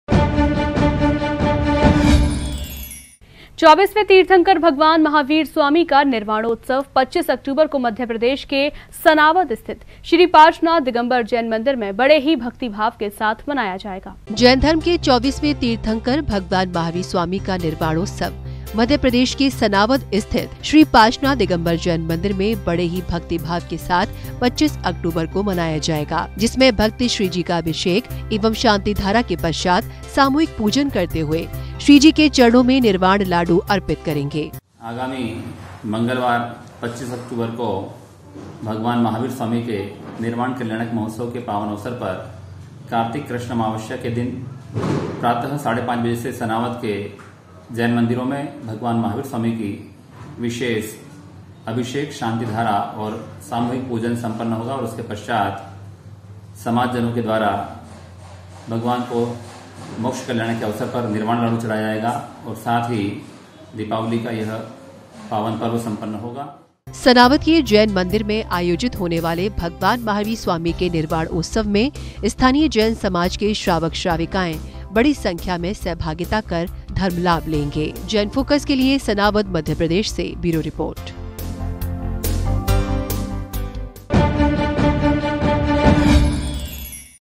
श्री पार्श्वनाथ दिगम्बर जैन मन्दिर में होगा आयोजन | Sanawad News | JAIN FOCUS
मध्य प्रदेश के सनावद में निर्वाणोत्सव
श्री पार्श्वनाथ दिगम्बर जैन मन्दिर में होगा आयोजन
अभिषेक एवं शांतिधारा कर लेंगे धर्मलाभ
श्रावक- श्राविकाएं करेंगे सहभागिता
श्रीजी के चरणों में अर्पण करेंगे निर्वाण लाडू